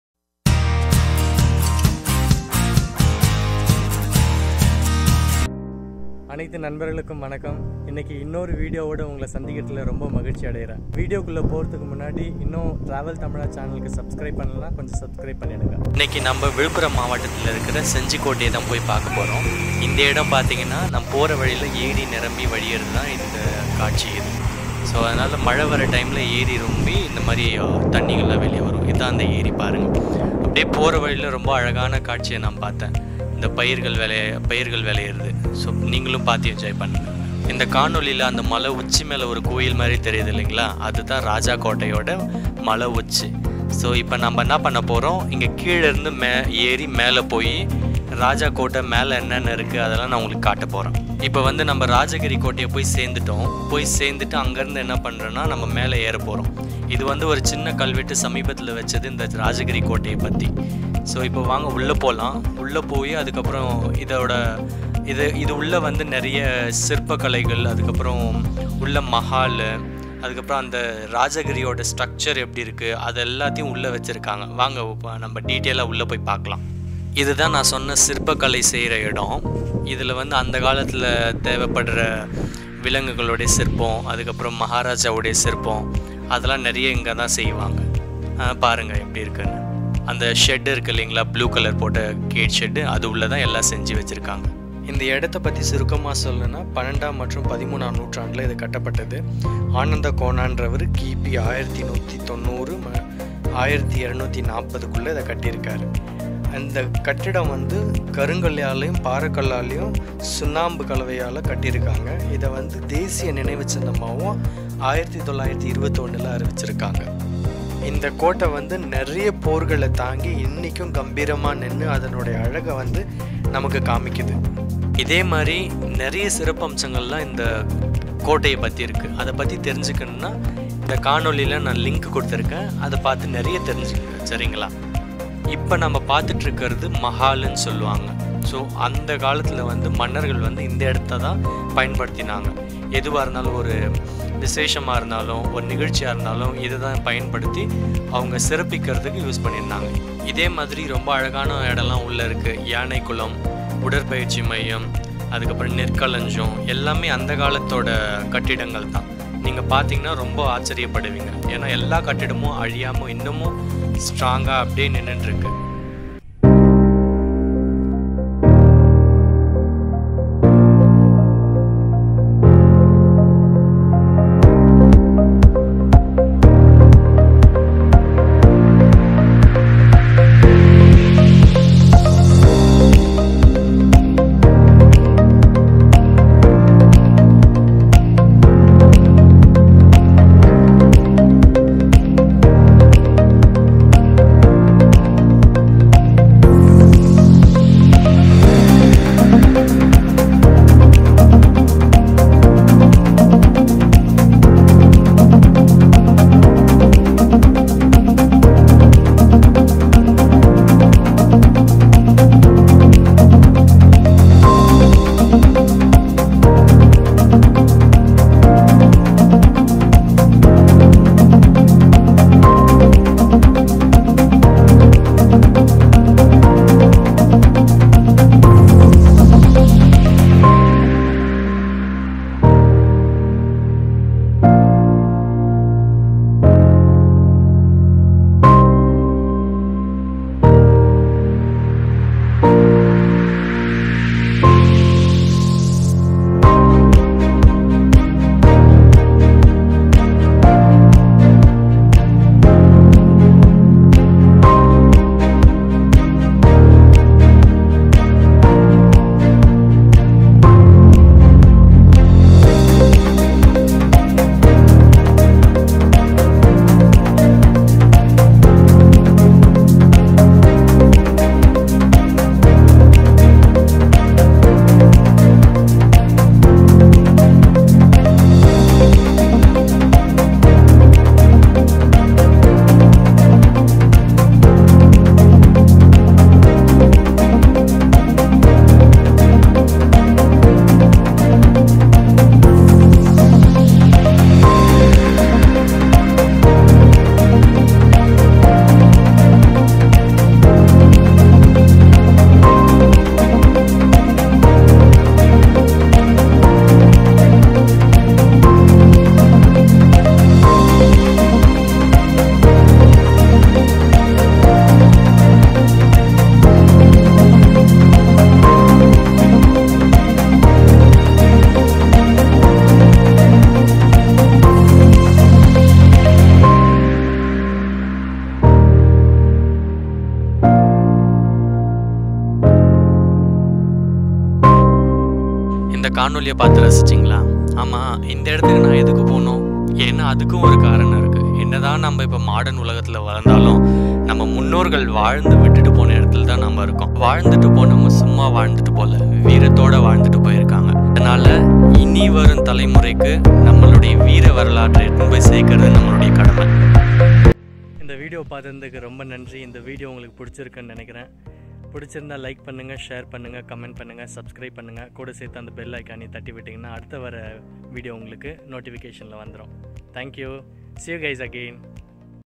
National Monument Genjee Fort (Rajagiri) | Historical Place in India | Must watchPlace in Tamil Nadu
Dear Friends,

This is about the most important and attractive Fort in India.